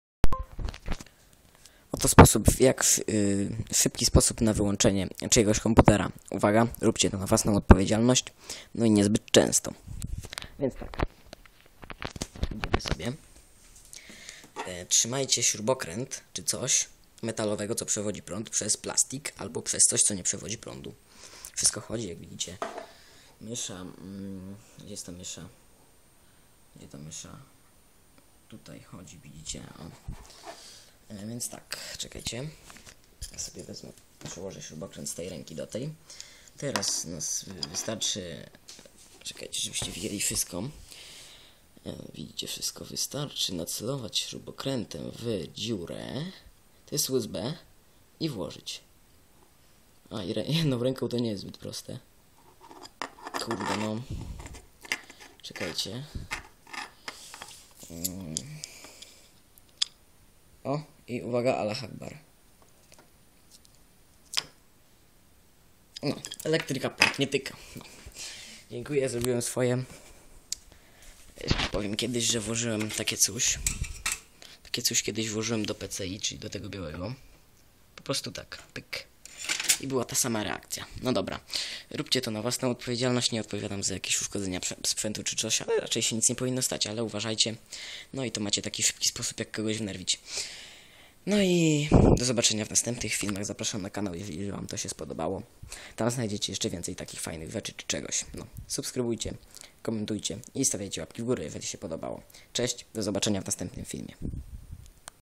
PORADNIK JAK W SZYBKI SPOSóB WYŁĄCZYĆ KOMPUTER WSADZAJĄC ŚRUBOKRĘT W USB :D
Jeżeli Wam się spodobało dajcie łapke suba i zostawcie po sobie ślad w postaci komętarza I niezapomnijcie zajżeć na kanał po więcej!